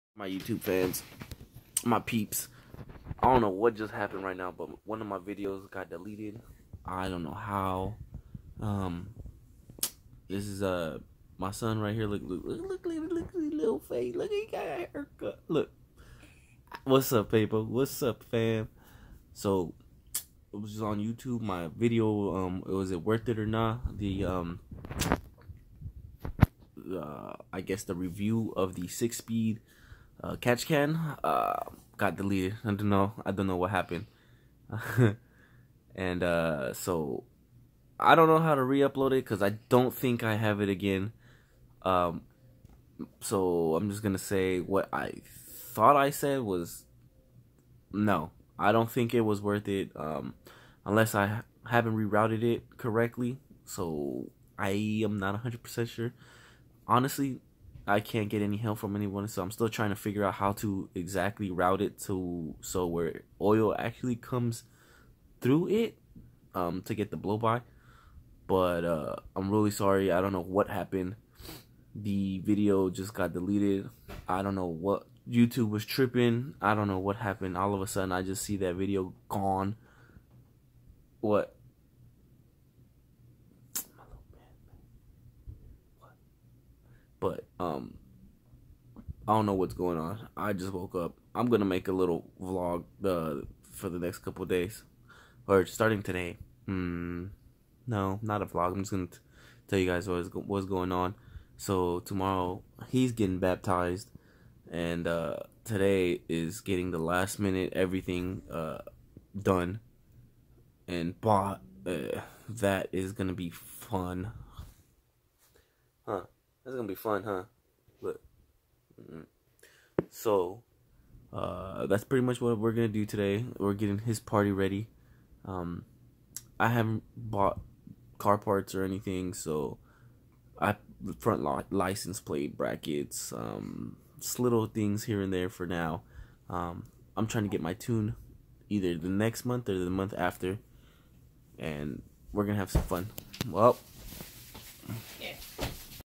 Youtube Acting Up.. Video Deleted
Sorry fam one of my videos was deleted somehow I will try to reupload it if I find it
Busy busy busy